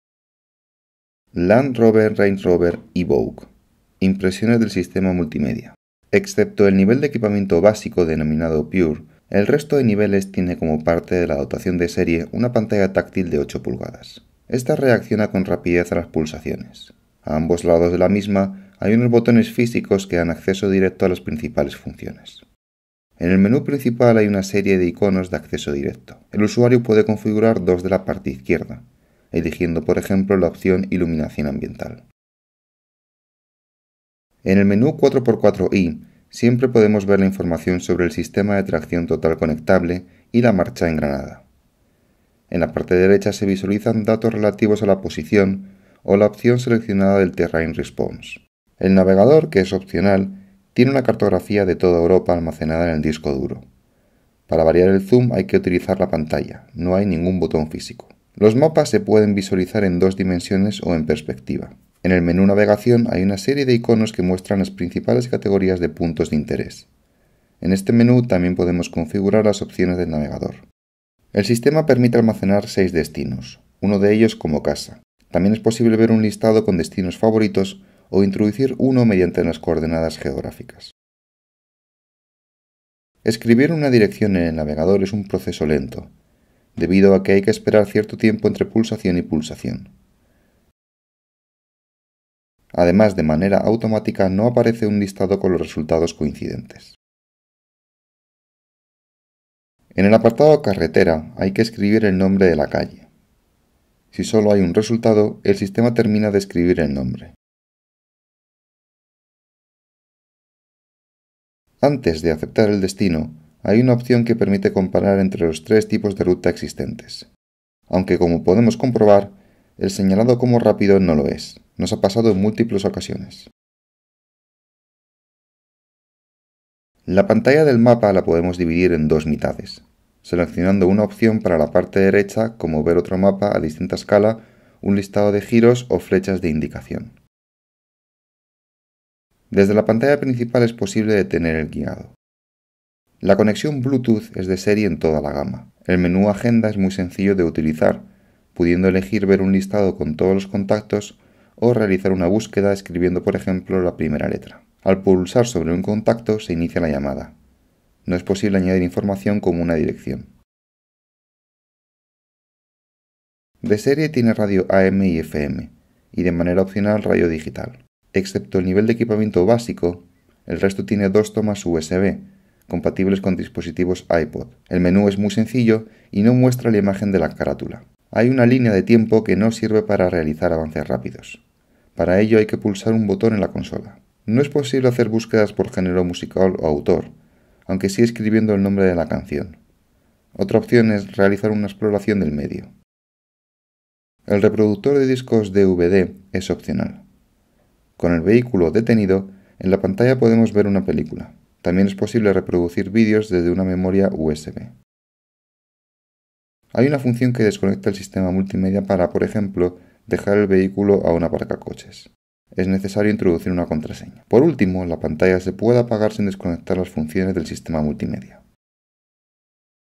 Land Rover Range Rover Evoque. Modelo 2014. Impresiones sistema multimedia
Los niveles «PureTech», «Dynamic» y «Prestige» tienen una pantalla a color de 8 pulgadas, radio FM, lector de discos CD, doble toma USB, conexión auxiliar de audio/vídeo y un sistema de sonido de la marca Meridiam con 11 altavoces. Opcionalmente pueden tener navegador, disco duro, lector de discos DVD y un sistema de órdenes vocales. Más información